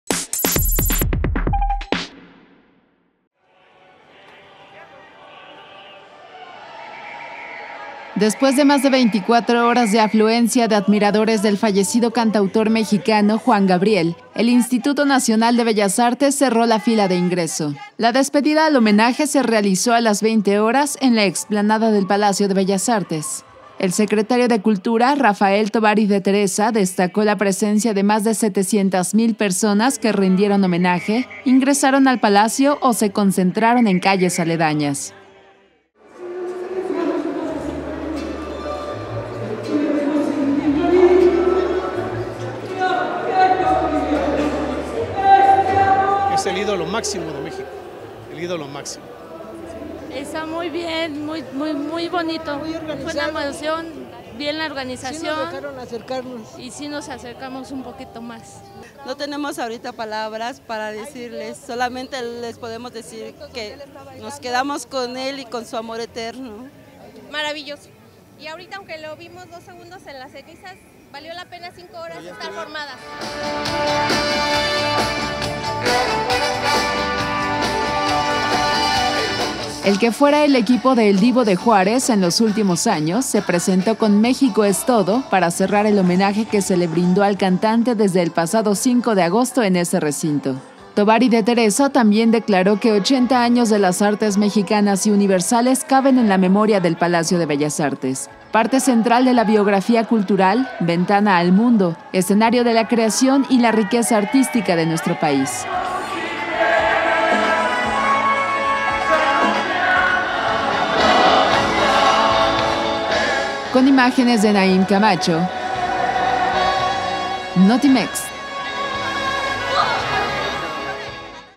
700 mil personas rindieron homenaje a Juan Gabriel en Bellas Artes
Después de más de 24 horas de afluencia de admiradores del fallecido cantautor mexicano, Juan Gabriel, el Instituto Nacional de Bellas Artes concluyó los homenajes ara despedir al 'Divo de Juárez'